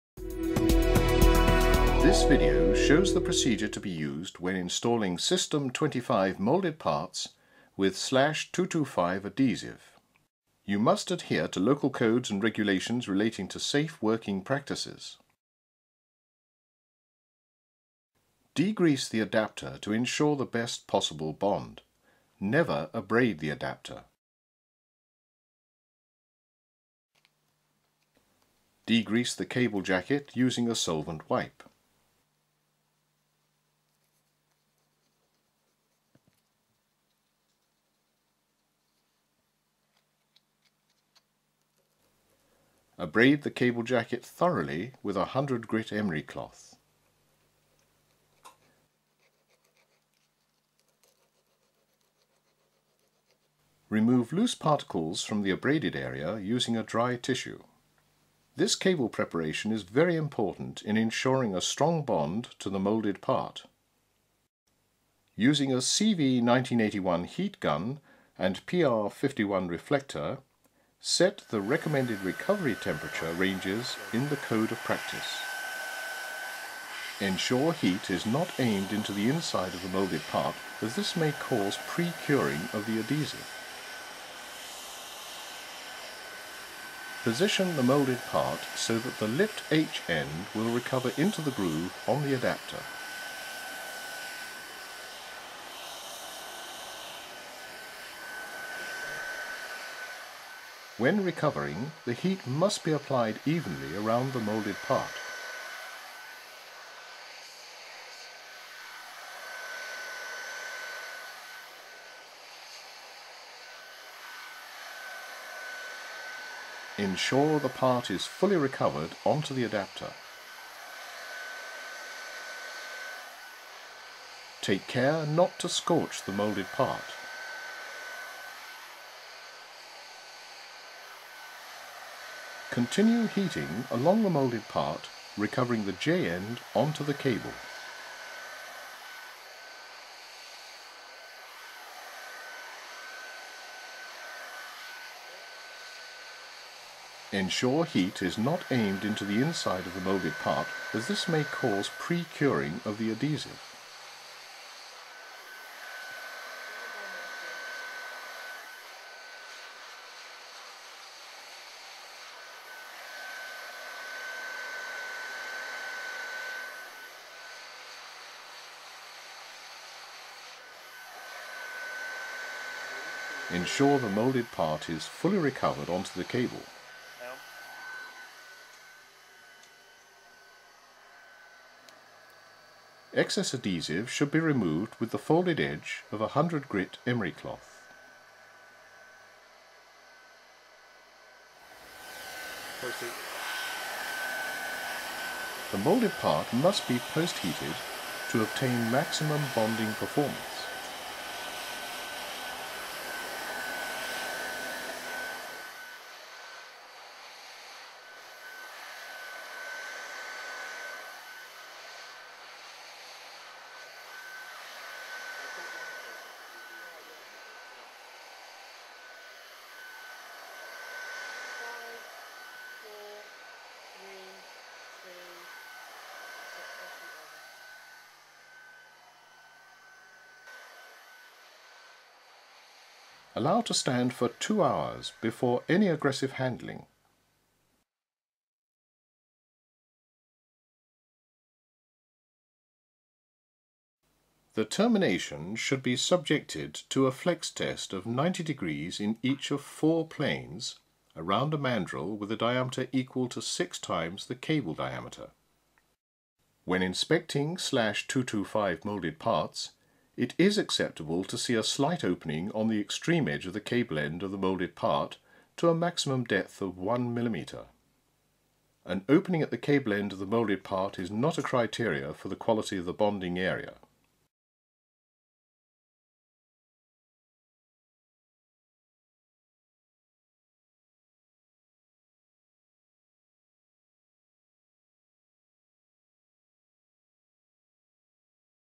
TE Connectivity: Installing System 25 Molded Parts with /225 Adhesive
How to install System 25 molded parts with /225 adhesive